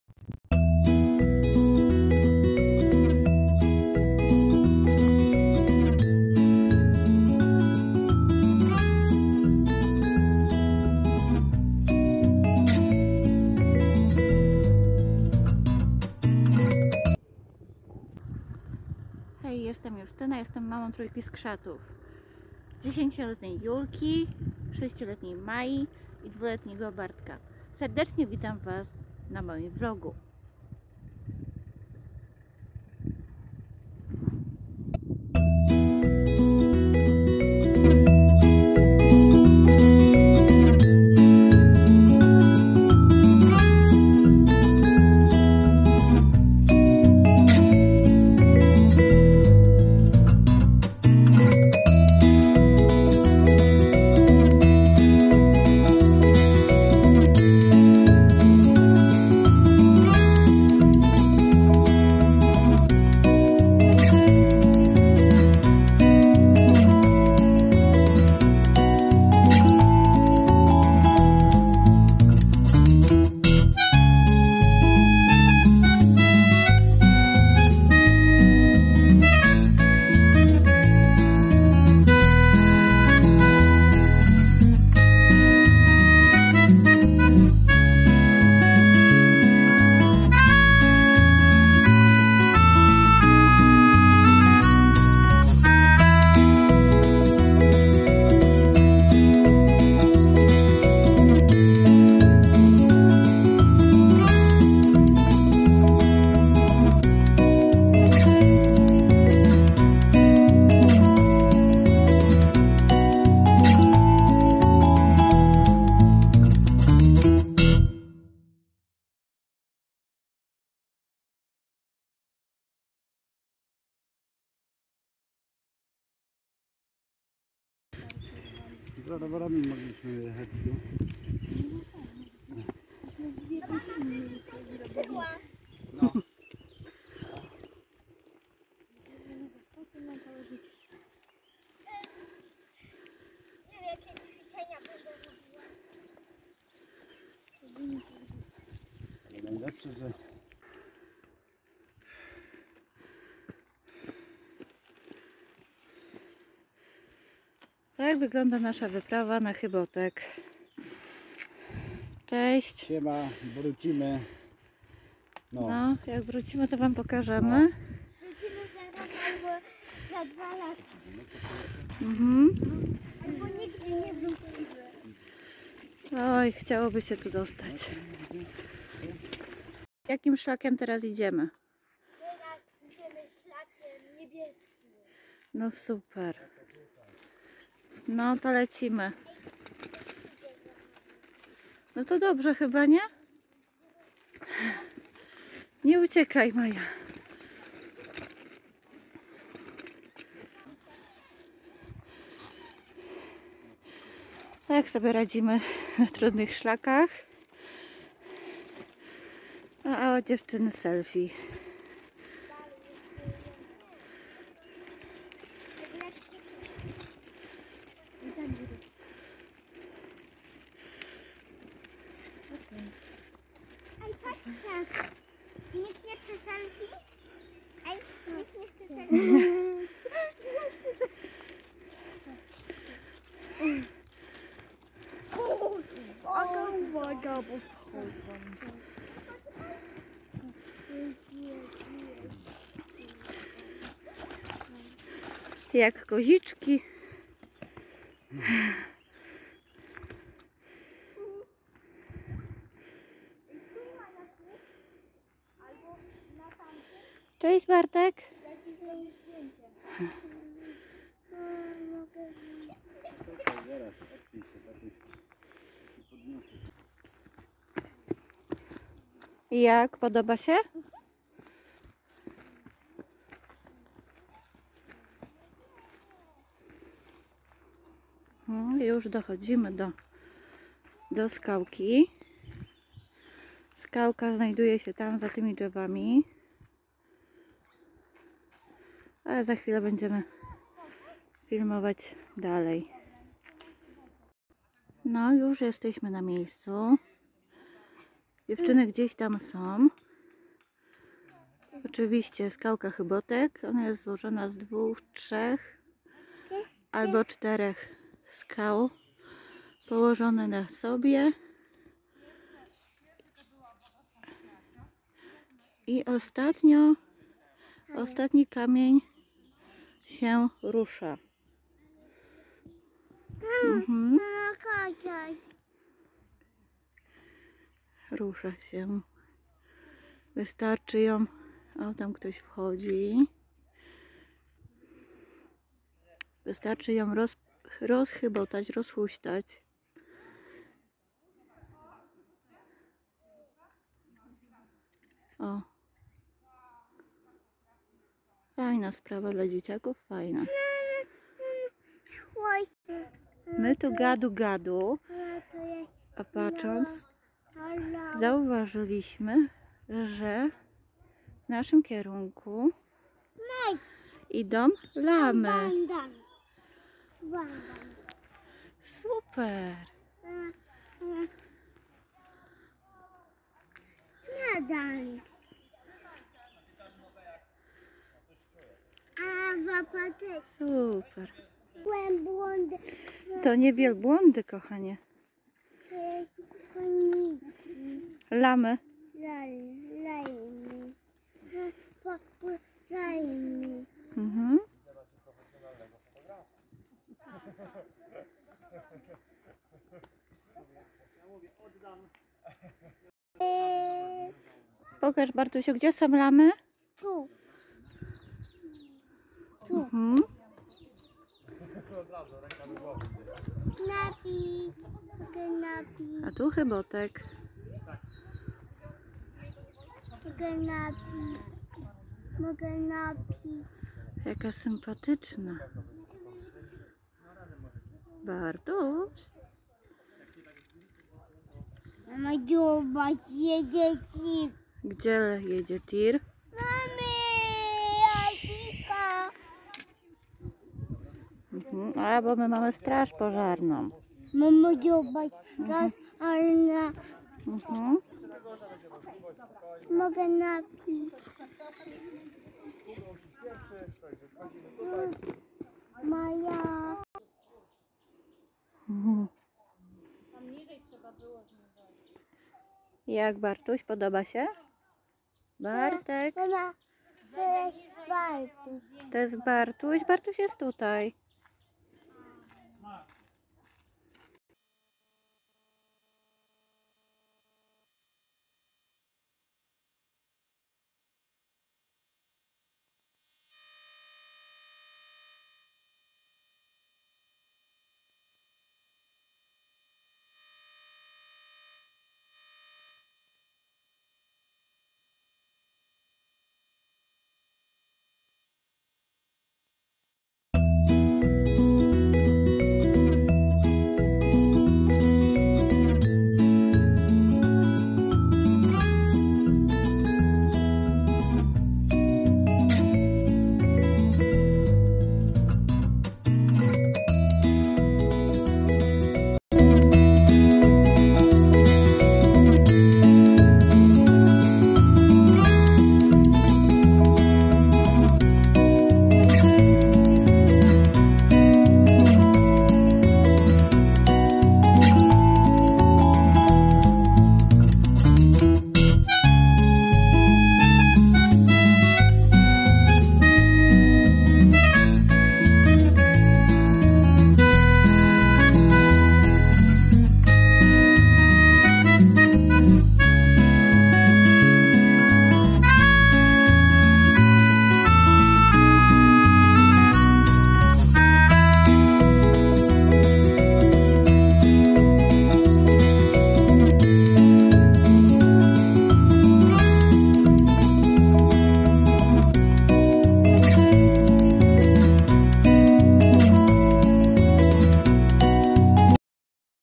Wędrówka na Chybotek (Góry Izerskie)
Hej. W tym filmiku zobaczcie nasze zmagania na szlaku (niwbieskim😂🤭) na Chybotek. Chybotek znajduje się na magicznymszlakuduchagór. Znajduje się on w Szklarskiej Porębie. Z Chybotka dziewczyny razem z tatą poszły na złoty widok - przepiękne miejsce (Ale to zostawiliśmy dla siebie). Zapraszamy na film!